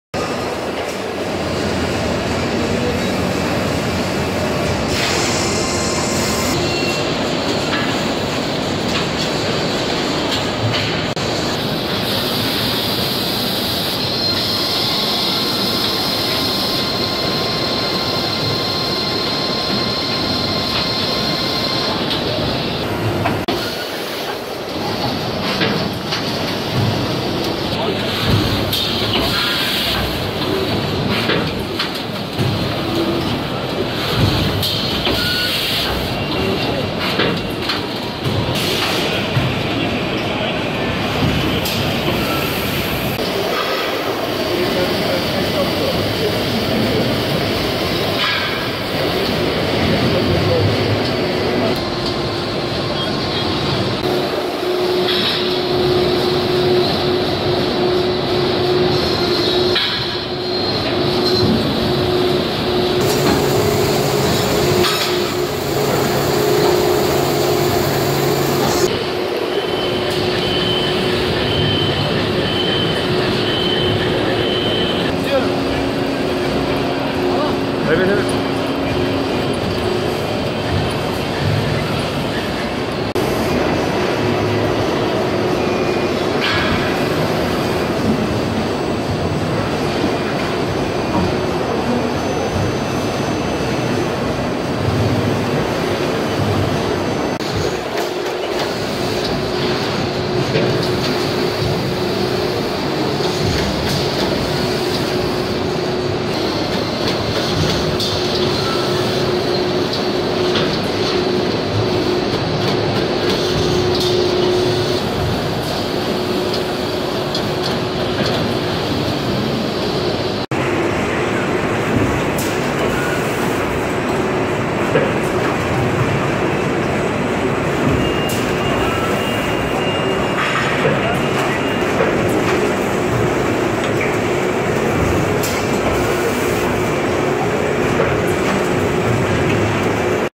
Ekin Makina Plast Eurasia 2016
7-10 Aralık 2016 tarihinde Plast Eurasia fuarında sergilediğimiz makinalar;

Ekin Speed Serisi 330T Yatay Enjeksiyon
Ekin Orion Serisi 380H-2C Döner Tabla Çift Renk Yatay Enjeksiyon
Ekin 270T Çift Renk Yatay Enjeksiyon
Ekin 600T Yatay Enjeksiyon
Ekin Orion Serisi 60T Yatay Enjeksiyon
Ekin 300V-2R Döner Tabla Dik Enjeksiyon
Ekin 55V-S Tek istasyon Kayar Tabla Dik Enjeksiyon